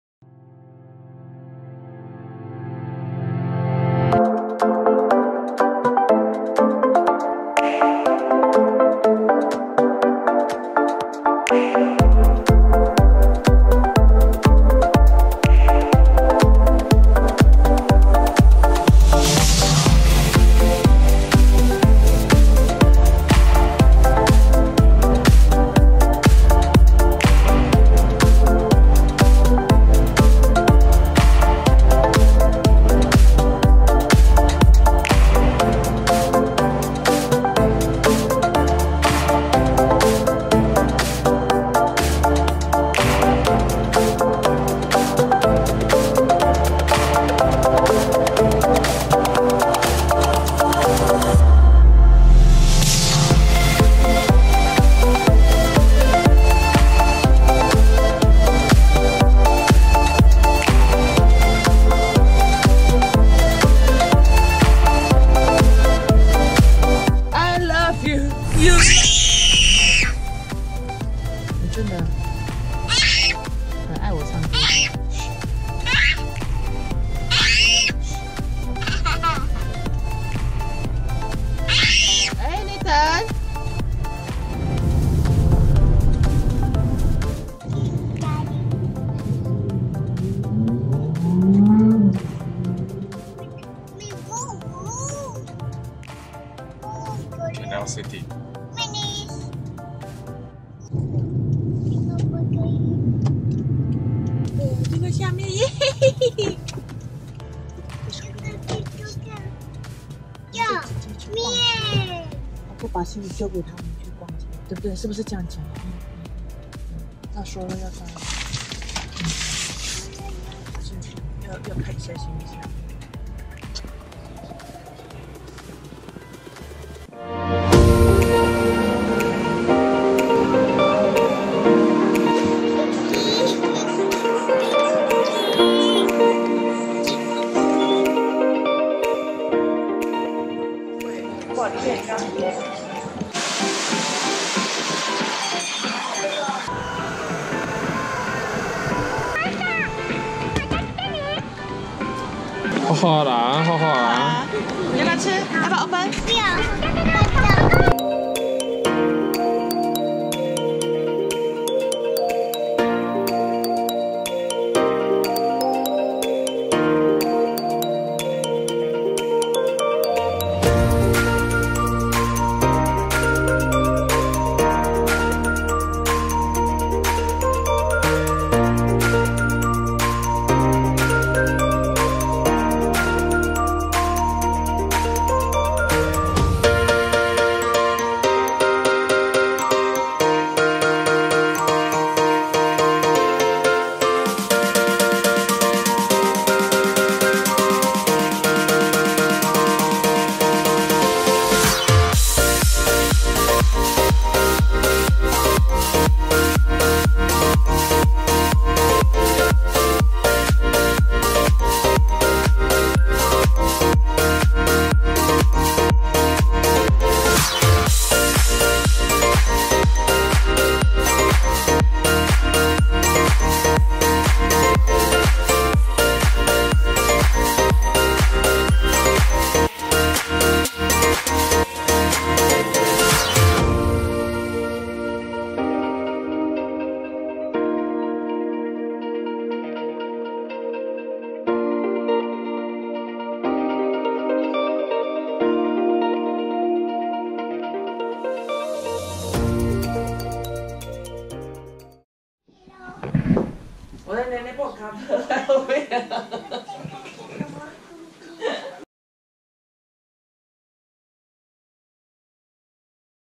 A foggy journey from Beppu City to Fukuoka (Episode 15.7)
Japan Road Trip: FOGGY drive from Beppu to Fukuoka (Episode 15.7)

We were returning to Fukuoka. A lot of bad weather driving today! The morning weather was very foggy, 20 meters visibility. It took us to drive almost 2 hours to get out of the fog. Canal City is great for shopping and foods.